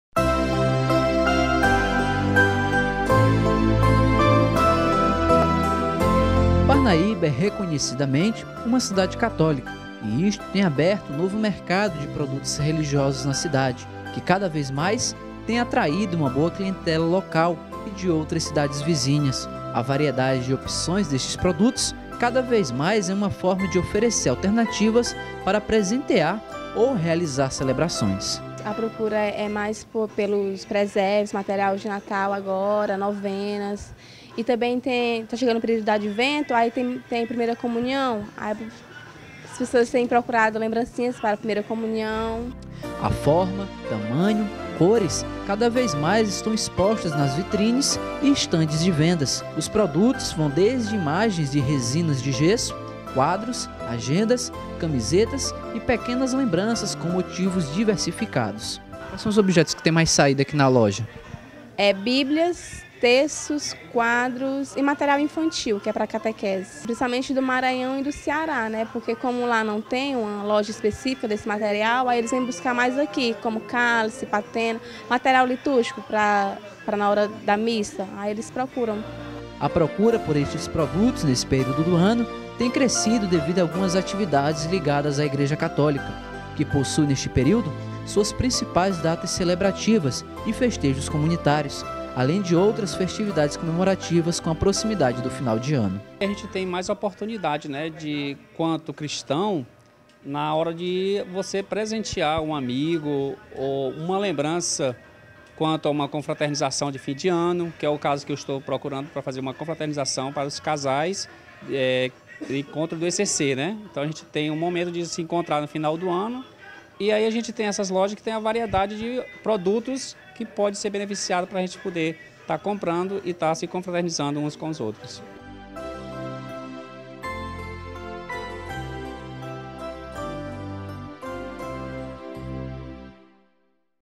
TV DELTA PARNAÍBA:ARTIGOS RELIGIOSOS
Parnaíba historicamente possui uma população tradicionalmente católica. para atender essa população um comercio especifico esta crescendo na cidade,  são locais de comercialização de produtos religiosos que possuem uma grande variedade  de  objetos que atende os mais variados gostos.